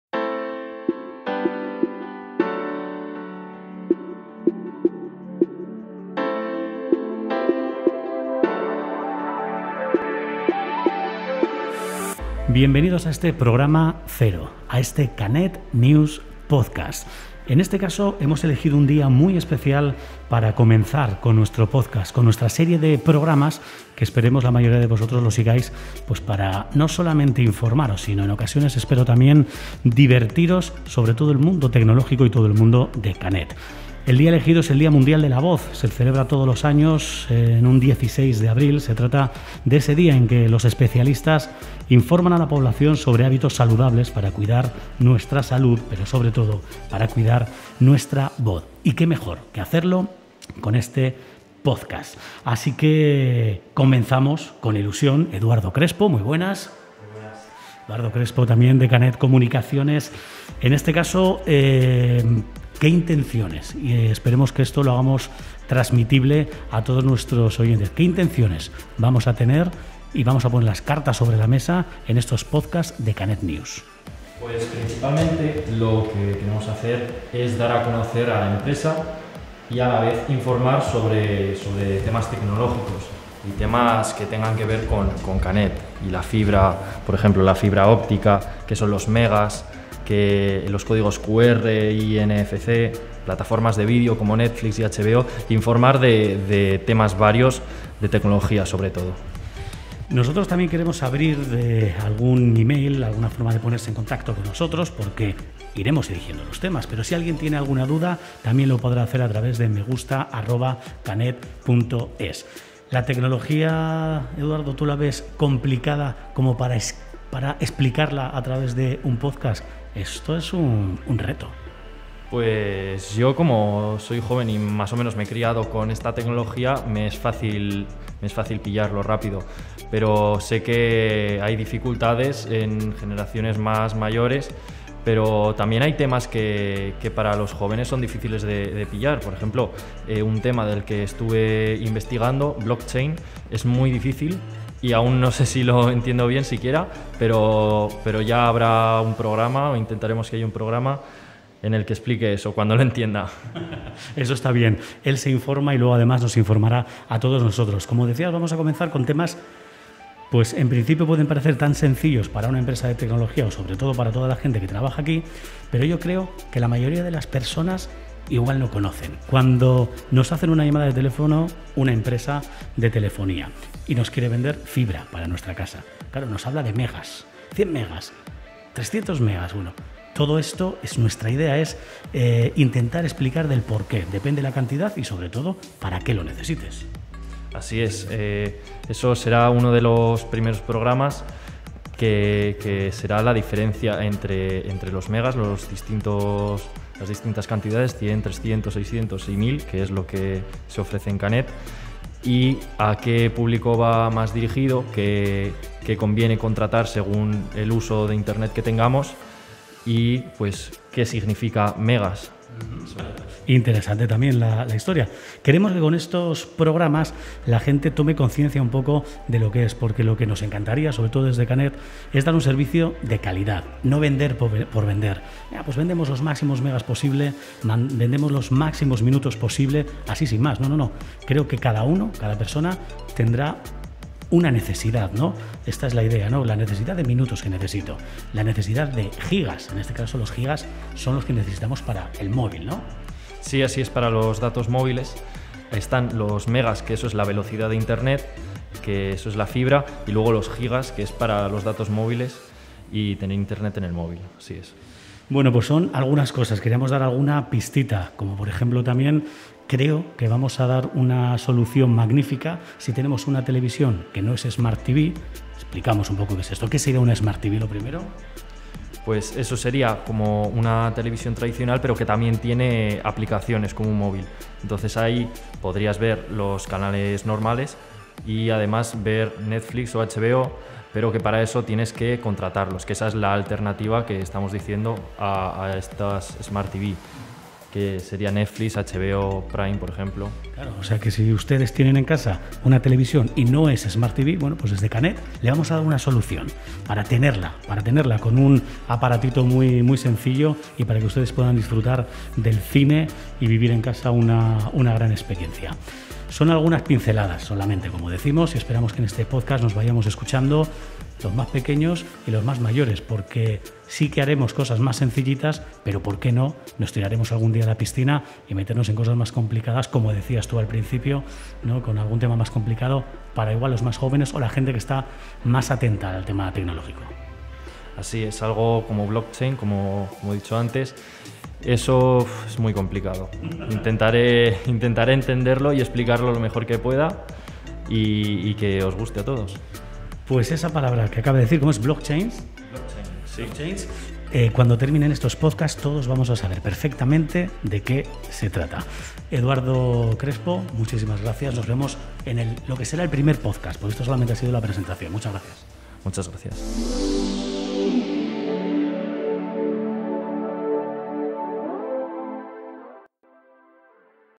Knet News Podcast Programa 0
Hoy es el Día de la Voz y estamos felices de presentaros el nuevo podcast de Knet, "Knet News Podcast".
Un espacio donde hablaremos principalmente sobre temas tecnológicos.
Subiremos un episodio semanal.